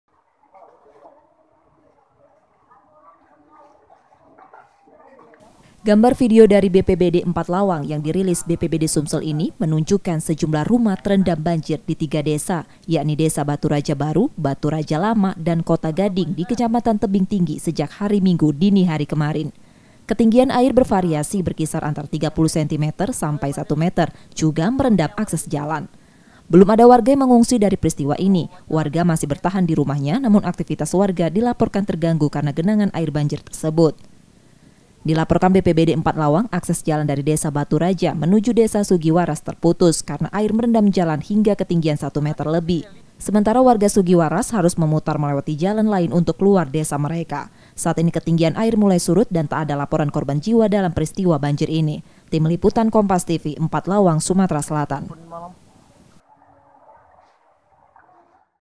3 Desa Di Kecamatan Tebing Tinggi Empat Lawang Banjir
Tiga Desa Di Kecamatan Tebing Tinggi, Empat Lawang Sumatera Selatan Terendam Banjir Sejak Hari Minggu Kemarin. Warga Yang Rumahnya Terendam Air Masih Bertahan Dan Belum Ada Yang Mengungsi.